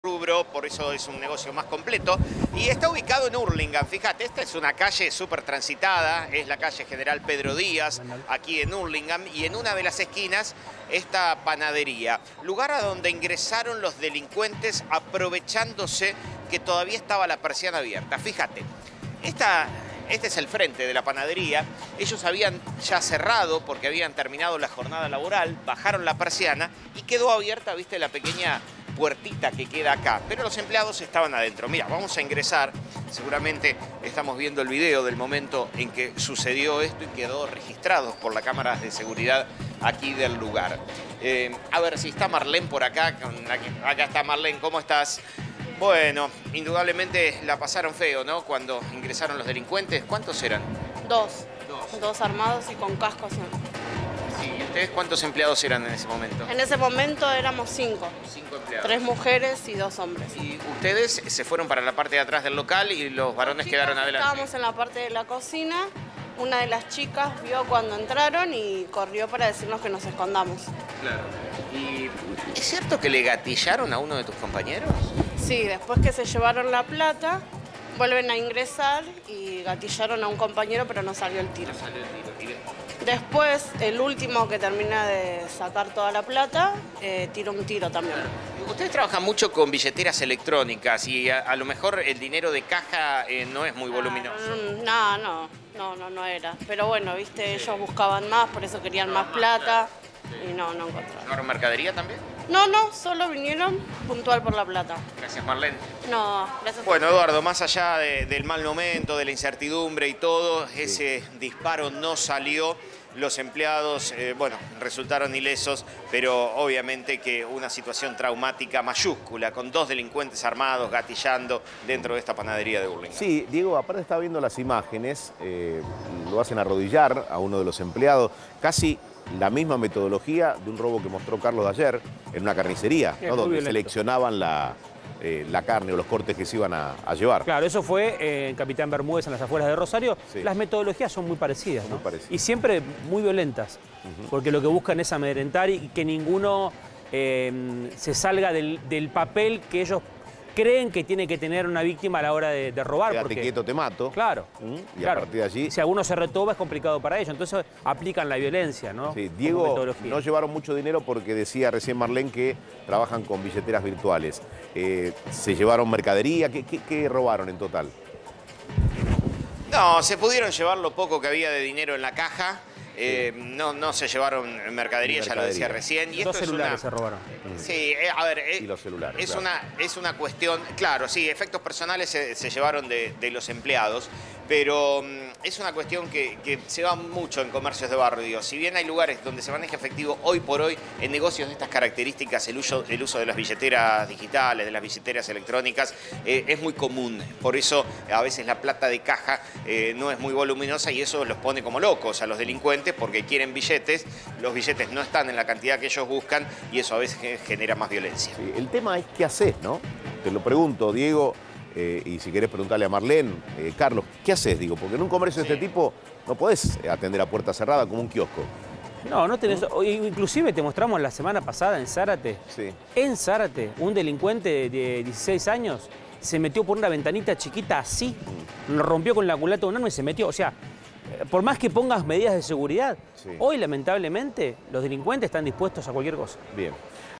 🚨 Violento robo a una panadería en Hurlingham
Dos delincuentes armados y con cascos asaltaron una panadería. Una vez que tenían lo recaudado, los ladrones le gatillaron a uno de los empleados.